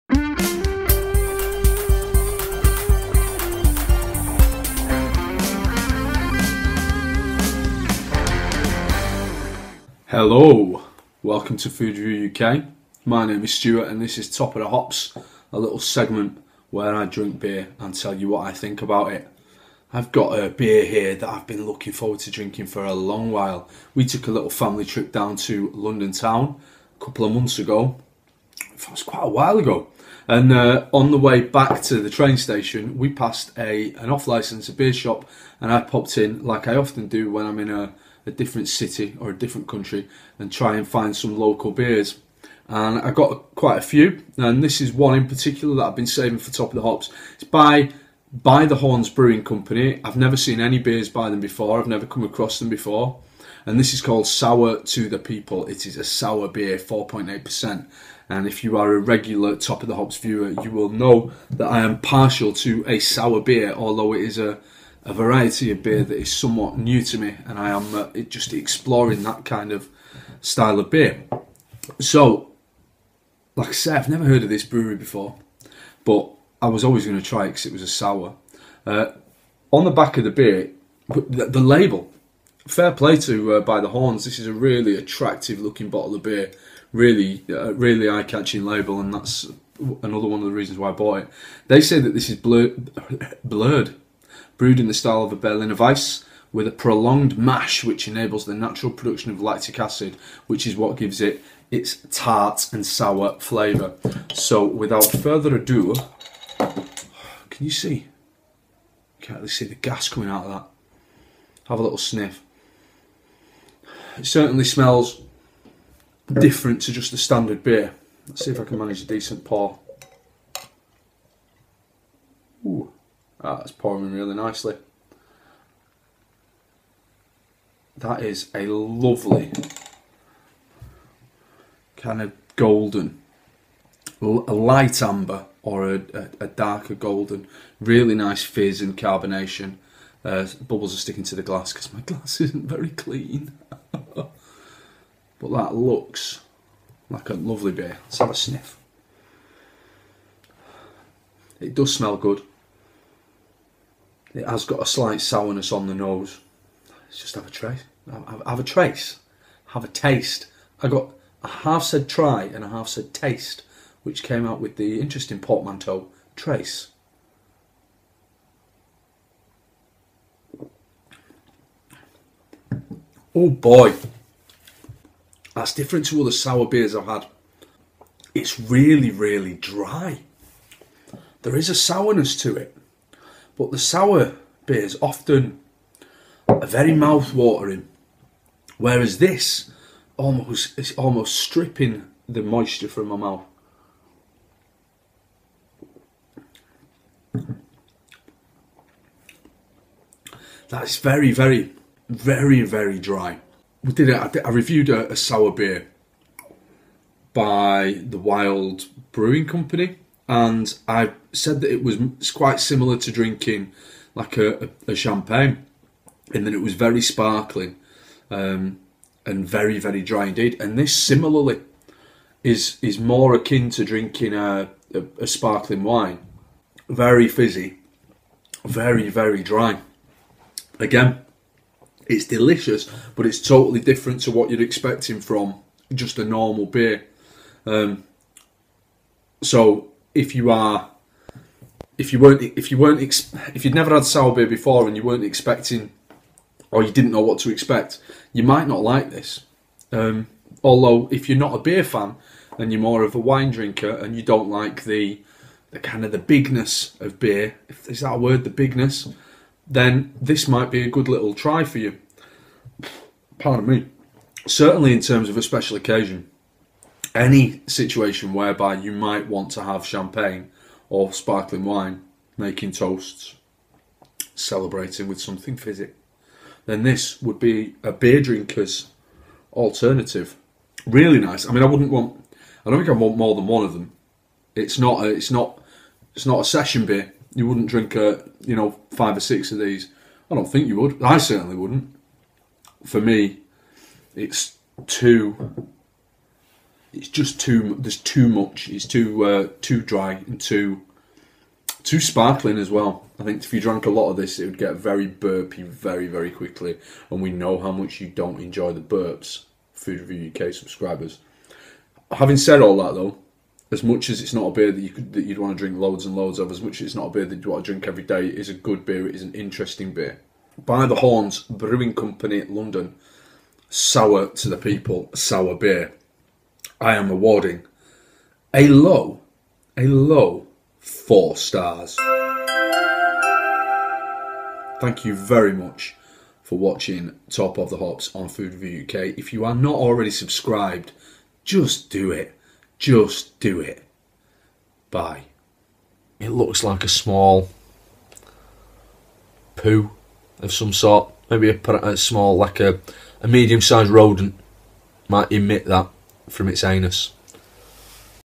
Sour To The People Review | By The Horns Brewing Co
Welcome back to Top Of The Hops, and this time Stu is going to give power to the people with a sour beer from the By The Horns Brewing Company.